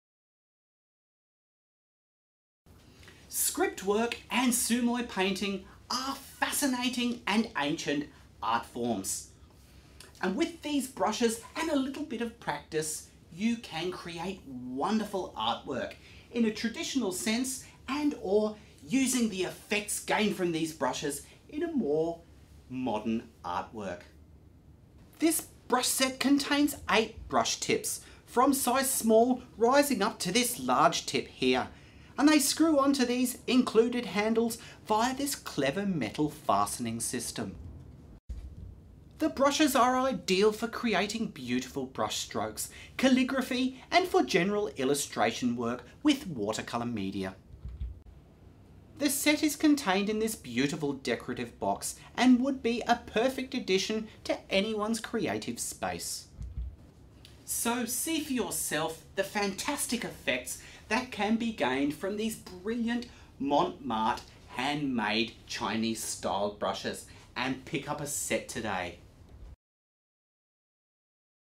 Demo - Chinese Calligraphy Brush Set - MAA0006
General Usage
 Create beautiful traditional Chinese paintings and calligraphy with this elegant gift box set.  This set contains 8 different sized screw on brush tips and 2 wooden brush handles.  Because of the brush's fantastic water holding capacity these brushes are also suitable for use with watercolours.